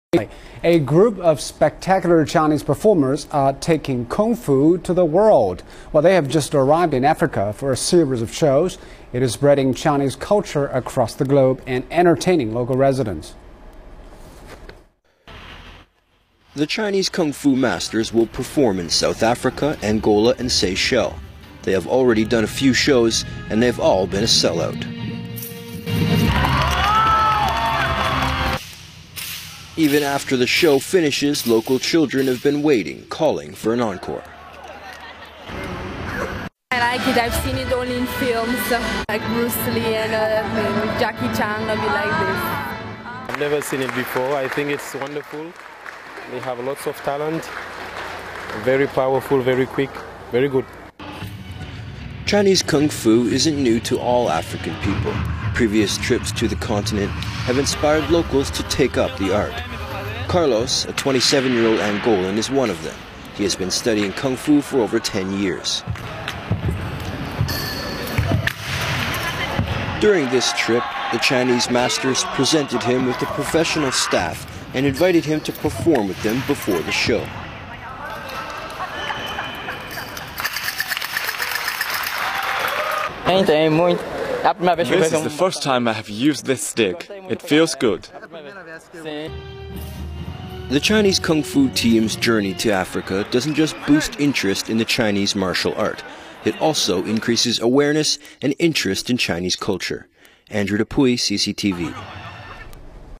Chinese Kungfu wows Africa audience
A group of spectacular Chinese performers are taking Kungfu to the world.  The Chinese kungfu masters will perform in South Africa, Angola and Seychelles. An audience member said, "I know about Chinese kungfu from movies. But the masters' performance is very different to that in the movies."